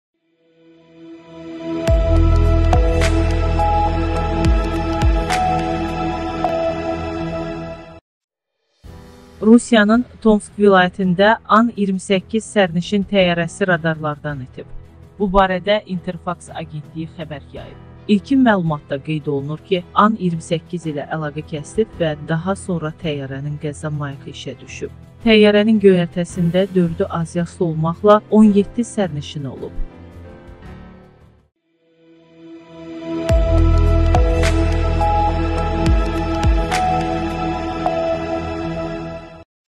TƏCİLİ! Sərnişin təyyarəsi radarlardan itib
Rusiyanın Tomsk vilayətində An-28 sərnişin təyyarəsi radarlardan itib.

Bu barədə “İnterfaks” agentliyi xəbər yayıb.

İlkin məlumatda qeyd olunur ki, An-28 ilə əlaqə kəsilib və daha sonra təyyarənin qəza mayakı işə düşüb.

Təyyarənin göyərtəsində dördü azyaşlı olmaqla 17 sərnişin olub.